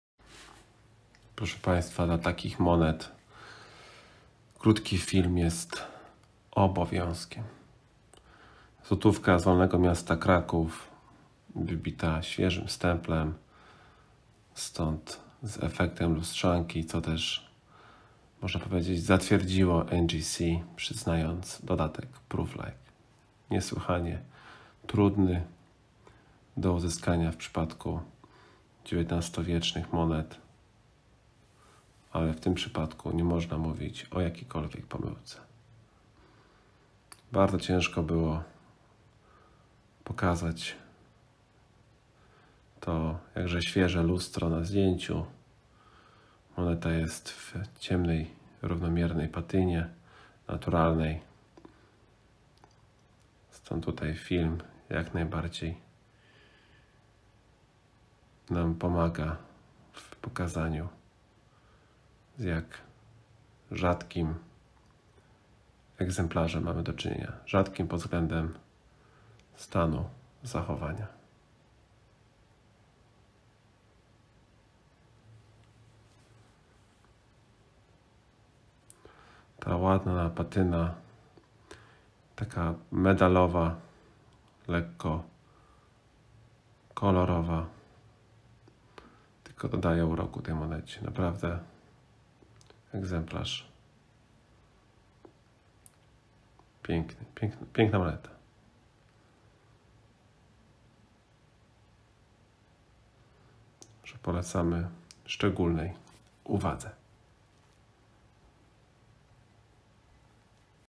Wolne Miasto Kraków, 1 złoty 1835 - NGC MS62 PL - jak lustrzanka - RZADKOŚĆ
Mennicza złotówka krakowska z efektem PROOF LIKE.

Piękna moneta z połyskiem jak lustrzanka w ładnej patynie.

Spektakularna prezencja.

Prawdziwa rzadkość w tym stanie zachowania.